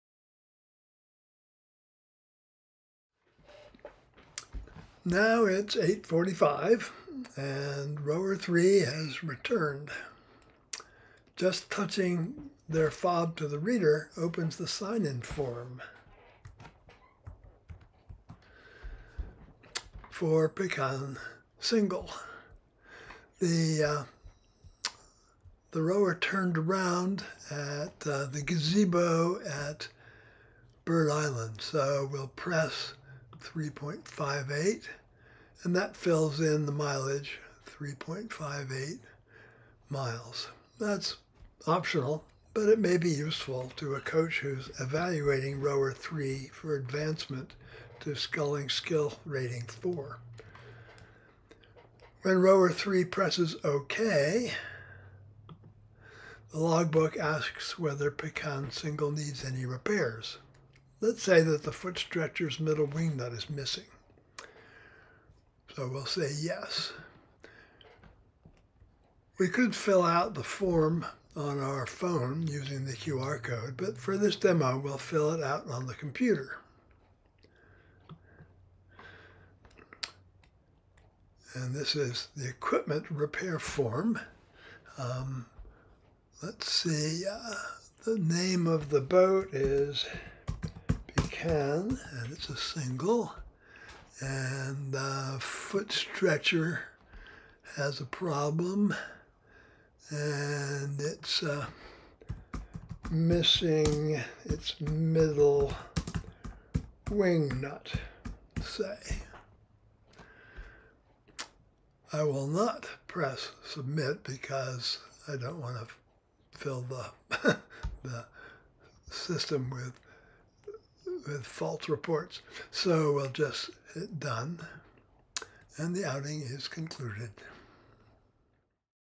Signing a boat in
Demonstrates how you sign a boat in after an outing. Included are options of recording the distance you rowed and reporting any repairs the boat needs.